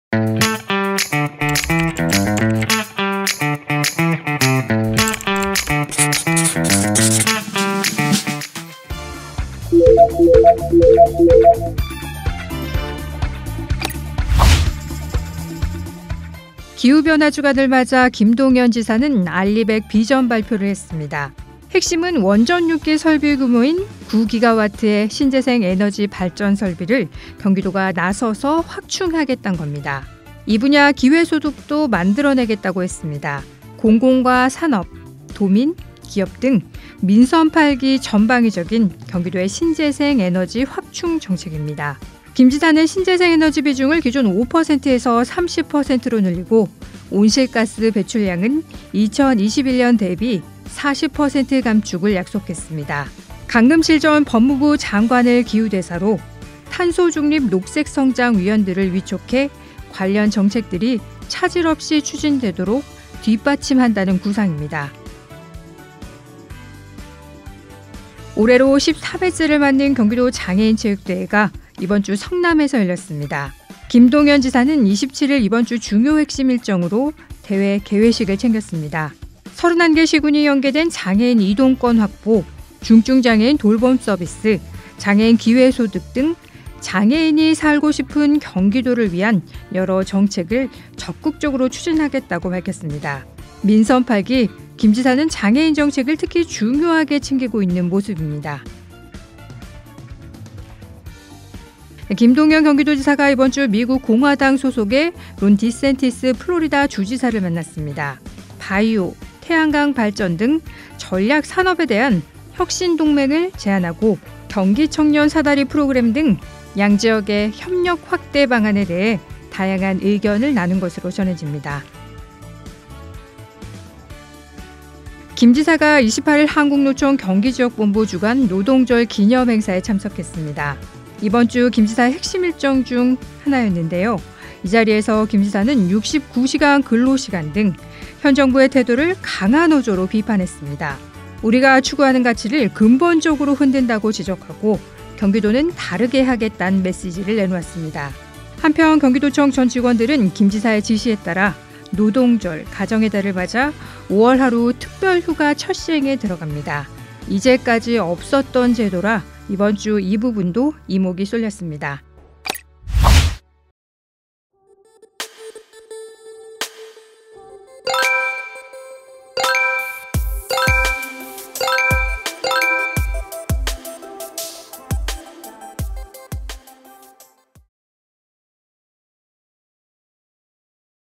4월 마지막 주 주간브리핑
4월 마지막 주, 경기도 주요 뉴스 간추려 전해드립니다.

[자막]
1.원전 6기 대체할 ‘경기 신재생에너지 정책’ 나왔다
기후변화주간을 맞아 김동연 지사는 ‘RE100 비전’ 발표를 했습니다. 핵심은 원전 6기 설비 규모인 9GW의 신재생에너지 발전설비를 경기도가 나서서 확충하겠다는 겁니다. 이 분야 기회소득도 만들어 내겠다고 했습니다. 공공과 산업, 도민, 기업 등 민선 8기 전방위적인 경기도의 신재생에너지 확충 정책입니다. 김 지사는 신재생에너지 비중을 기존 5%에서 30%로 늘리고, 온실가스 배출량은 2021년 대비 40% 감축을 약속했습니다. 강금실 전 법무부 장관을 기후대사로, 탄소중립 녹색성장 위원들을 위촉해 관련 정책들이 차질 없이 추진되도록 뒷받침한다는 구상입니다.

2. ‘장애인이 살고 싶은 도시’ 핵심 정책으로…
올해로 13회째를 맞는 경기도장애인체육대회가 이번 주 성남에서 열렸습니다. 김동연 지사는 27일, 이번 주 중요 핵심 일정으로 대회 개회식을 챙겼습니다. 31개 시·군이 연계된 장애인 이동권 확보, 중증장애인 돌봄서비스, 장애인 기회소득 등 ‘장애인이 살고 싶은 경기도’를 위한 여러 정책을 적극적으로 추진하겠다고 밝혔습니다. 민선 8기 김 지사는 장애인 정책을 특히 중요하게 챙기고 있는 모습입니다.

3. 김 지사, 론 디샌티스 美플로리다 주지사 면담
김동연 경기도지사가 이번 주 미국 공화당 소속의 론 디샌티스 플로리다 주지사를 만났습니다. 바이오, 태양광 발전 등 전략산업에 대한 ‘혁신 동맹’을 제안하고 ‘경기청년사다리프로그램(청년 사다리)’ 등 양 지역의 협력 확대 방안에 대해 다양한 의견을 나눈 것으로 전해집니다.

4. “노동에 대한 정부 태도와 경기도는 다르게 해보겠다”
김 지사가 28일 한국노총 경기지역본부 주관 노동절 기념행사에 참석했습니다. 이번 주 김 지사의 핵심 일정 중 하나였는데요. 이 자리에서 김 지사는 69시간 근로 시간 등 현 정부의 태도를 강한 어조로 비판했습니다. 우리가 추구하는 가치를 근본적으로 흔든다고 지적하고, 경기도는 다르게 하겠다는 메시지를 내놓았습니다. 한편, 경기도청 전 직원들은 김 지사의 지시에 따라 노동절·가정의 달을 맞아 5월 하루 특별휴가 첫 시행에 들어갑니다. 이제까지 없었던 제도라 이번 주 이 부분도 이목이 쏠렸습니다.

-김동연 “국익 사라져버린 한미정상회담. 근본적 원인은 리더십 위기”
-경기도, 개별공시지가 평균 5.32% 하락..14년 만에 하락세 전환
-경기도, 전국 최초 최중증 발달장애인 24시간 돌봄 실태조사
-‘경기청년 사다리 프로그램’, 31대 1 경쟁률..‘인기몰이’
-‘22~28일 기후변화주간’ 경기도 전역서 지구 환경 위해 노력